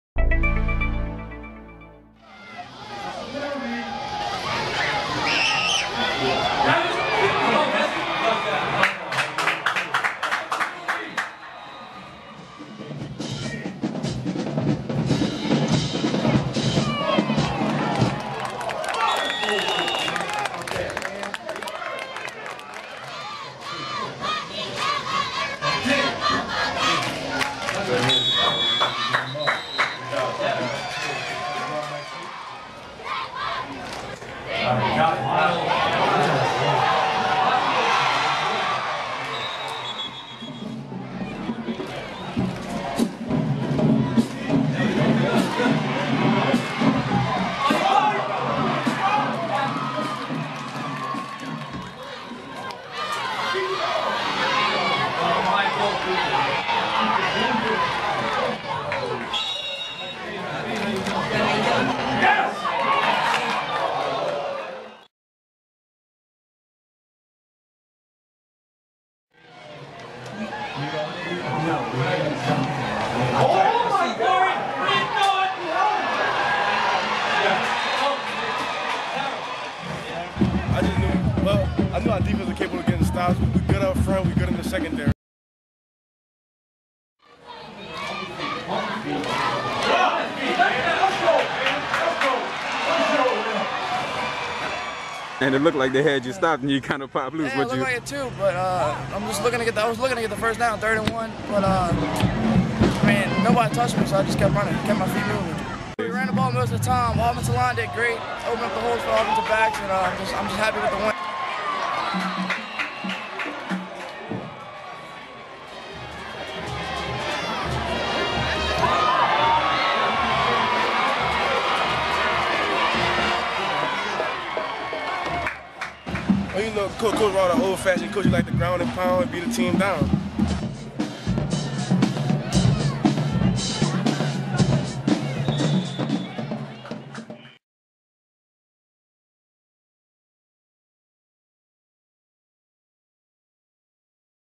Rummel rushes for four touchdowns, defense keeps East St. John scoreless: Video Highlights
Quarterback Chase Fourcade accounts for three touchdowns and Rummel defense keeps East St. John off the scoreboard.